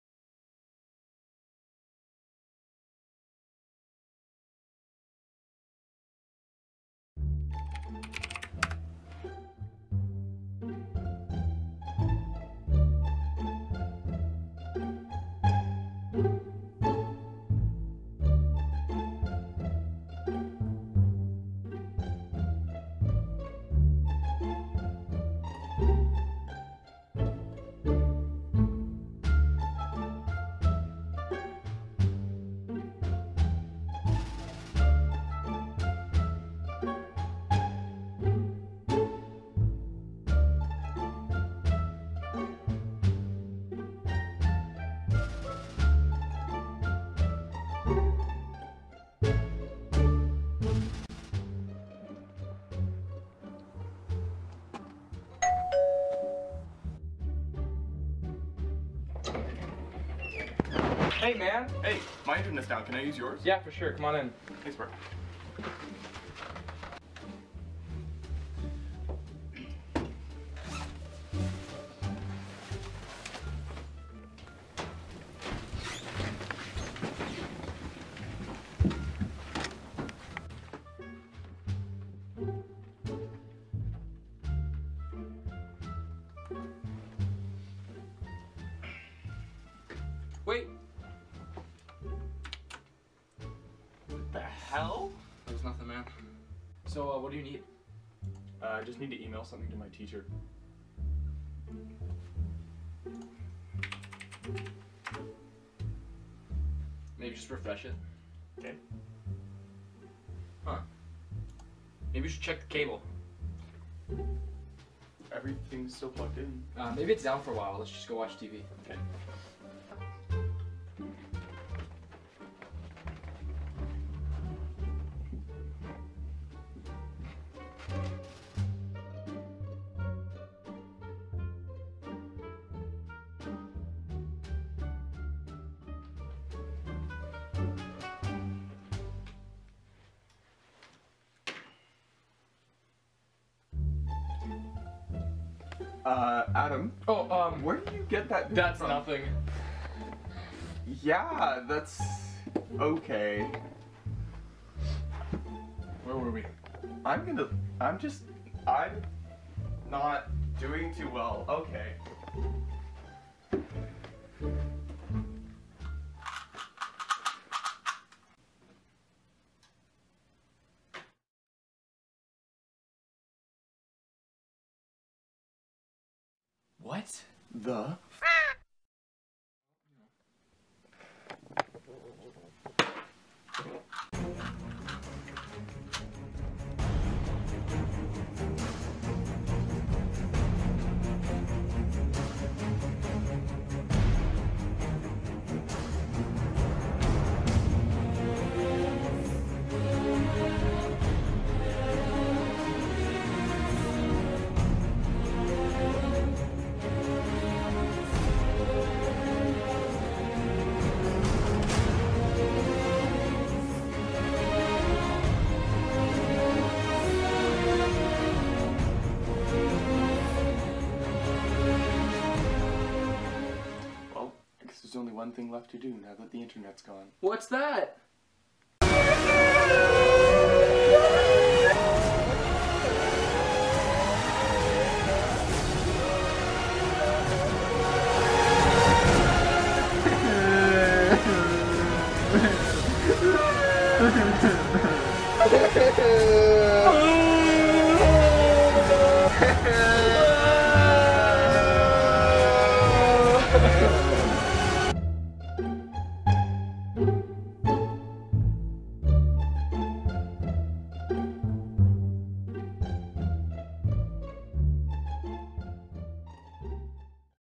Disconnected
What would it be like if the internet cut out...forever? Two teenage boys experience this disaster. Final project by Adam, Scott and Jamie.